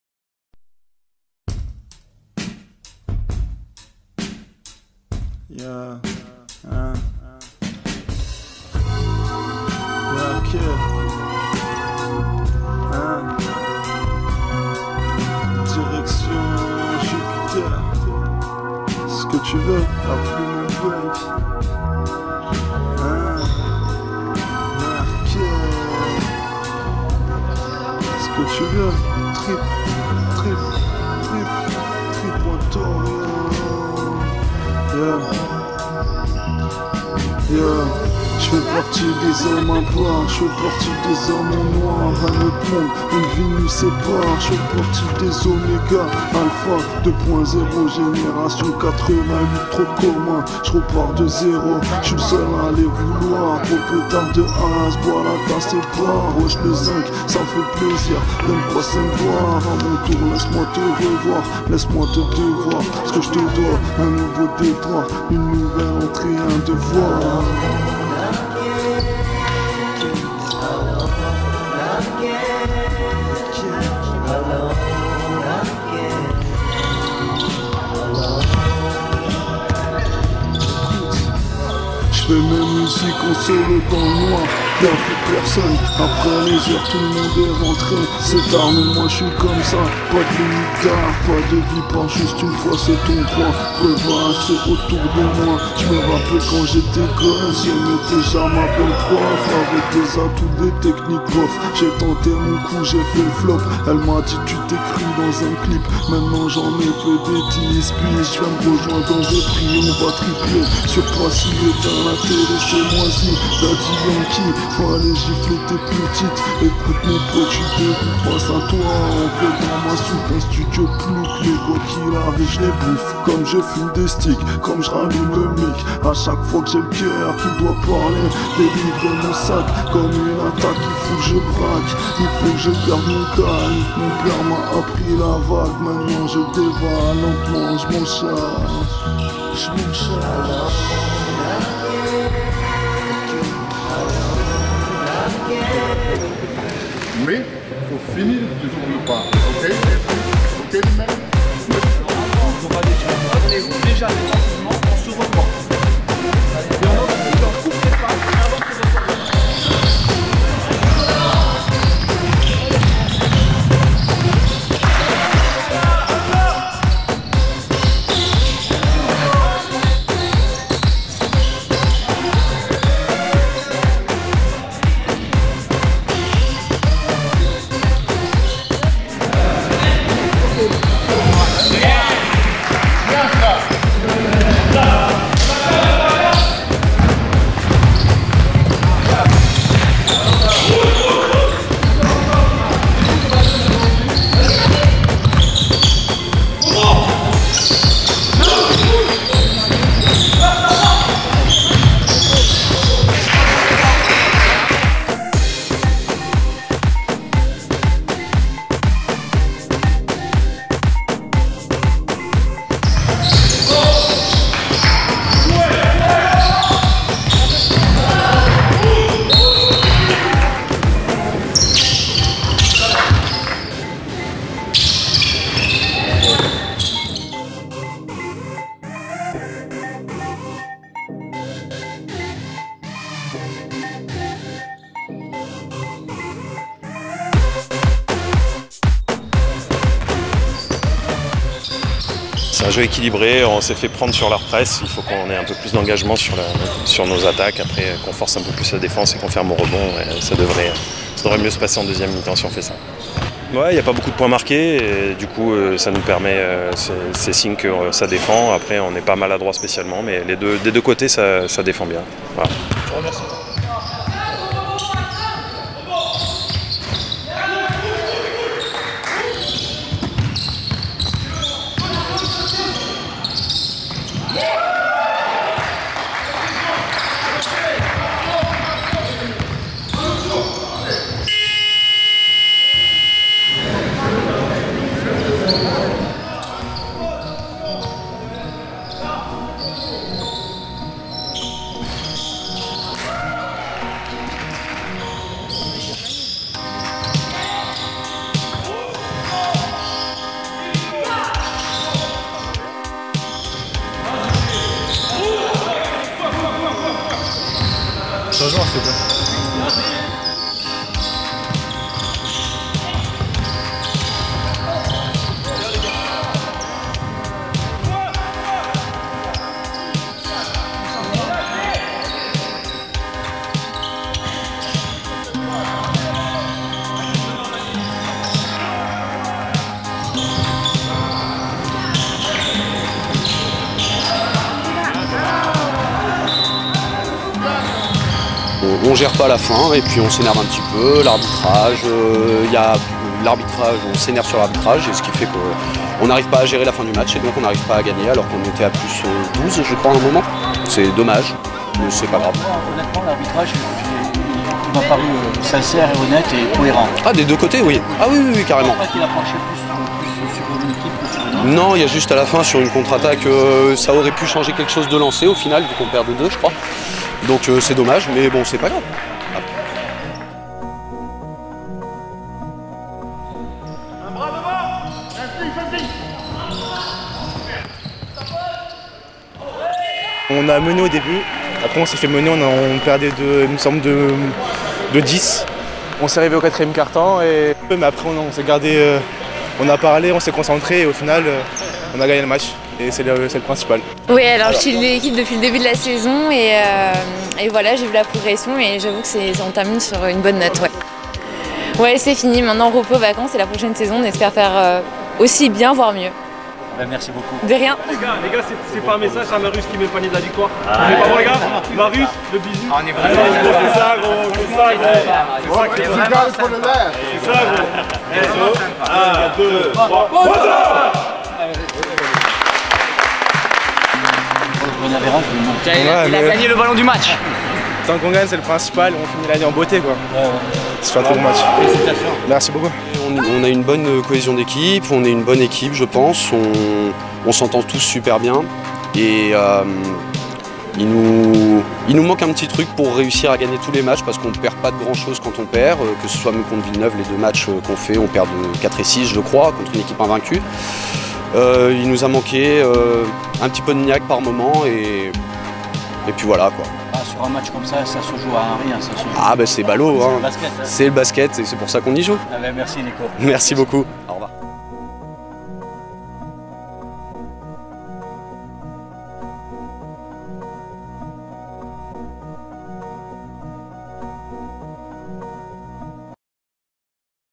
Pré Régionale Séniors Le Cannet VS NBAO 12.04.2018
En match avancé et le dernier de la saison. L'équipe du NBAO m'a donné rendez-vous à la salle MAILLAN. Les hommes de Lindsay HAIRSTON se sont imposés au bout du suspens (63 - 61)  jeudi soir, au terme d'une rencontre agréable et très disputée. Les Niçois du NBAO ont eu la balle de match entre les mains.